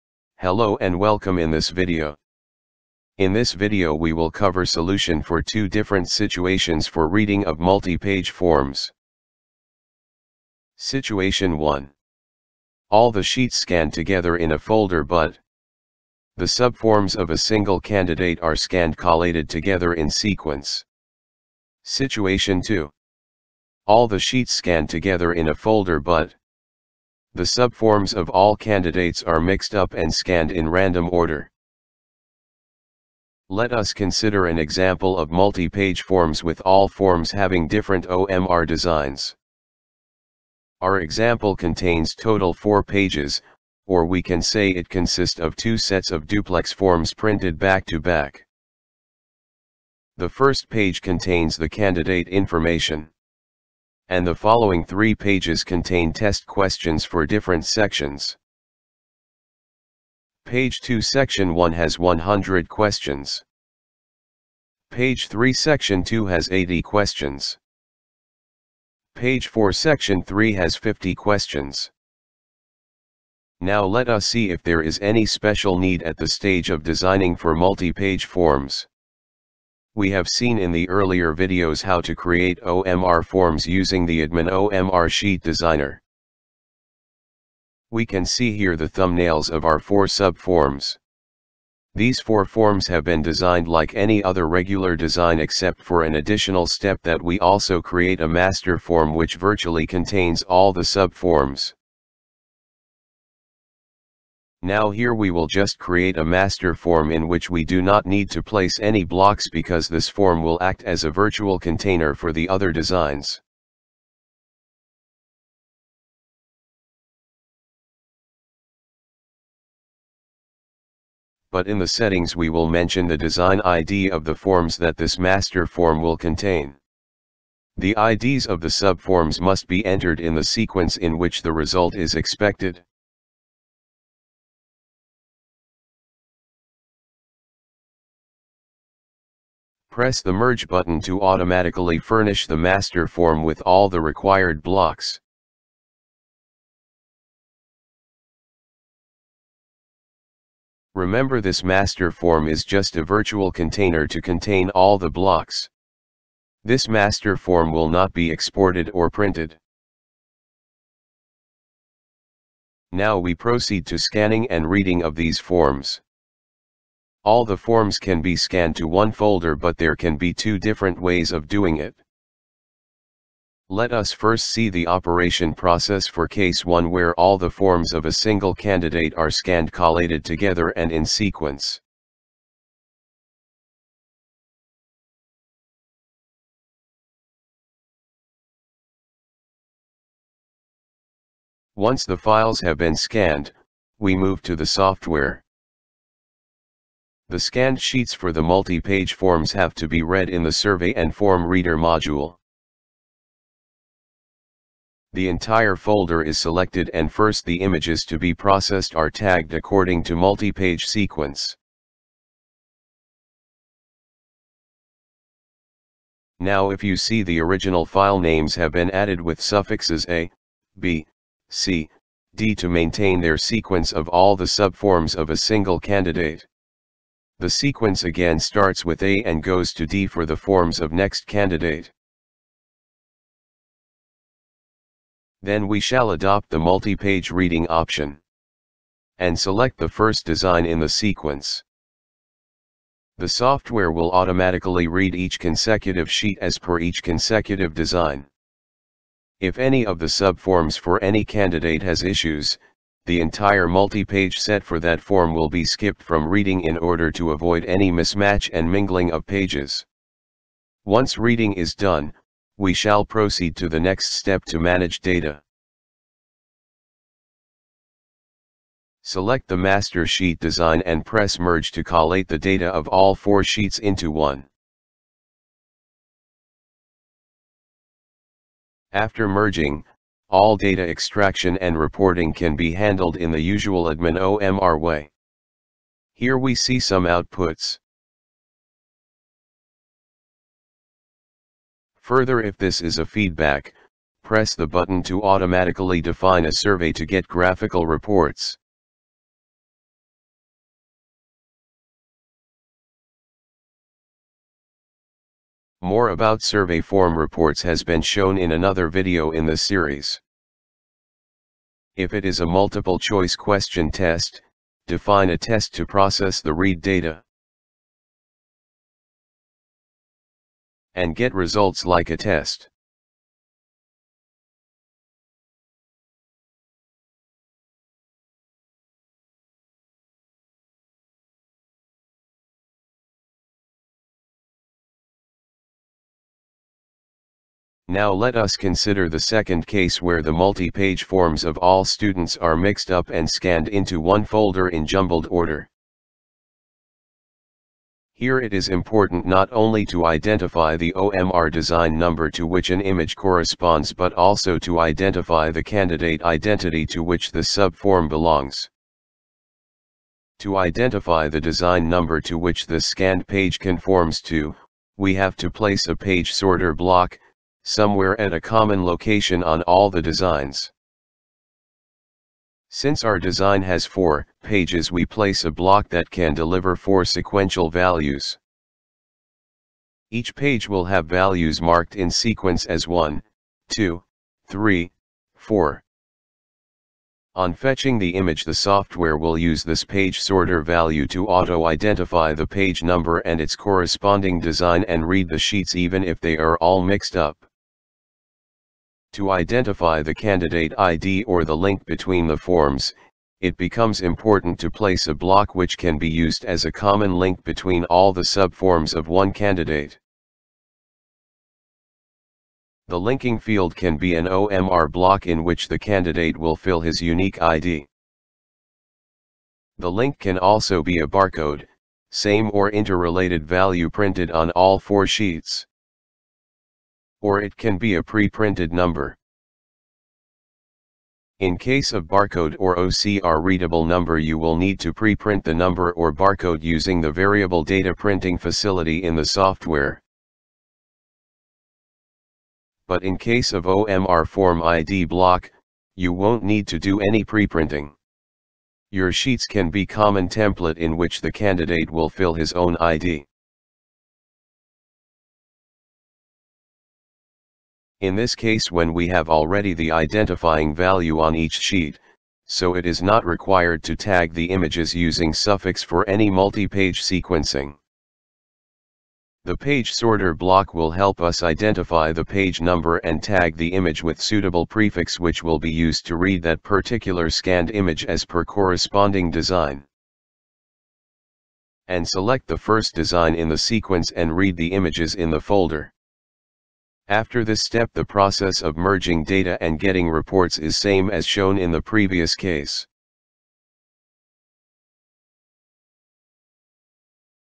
Solution for Processing of Multipage Forms using Addmen OMR Sheet Reader Software
Learn more about the Processing of Multipage Forms using Addmen OMR Sheet Reader Software.
OMR Form Reader is a quick and effective way of gathering and processing survey, feedback, and application form data using OMR Forms. OMR Form Reader is a special functionality unit of Addmen OMR Sheet Reader Software which is used for reading of Multipage Forms
In this video we have shown the workings of Processing of Multipage Forms using Addmen OMR Sheet Reader Software. You will get all the information regarding the modules and features of the OMR sheet reader software